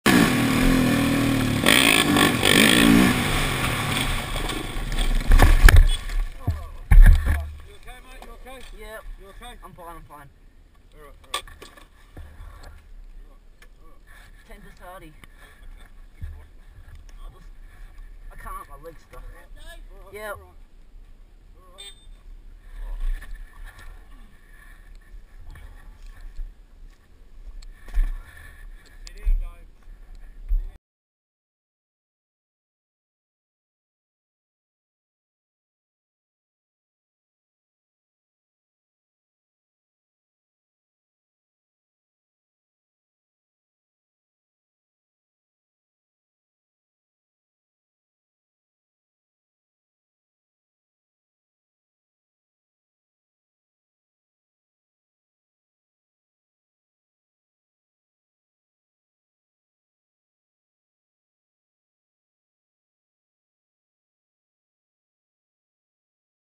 sardie and dave bad crash 15 09 2013
Going down a track and the Honda's wheel slipped out from under it and sent the bike tumbling and i got stuck to deal with the aftermath.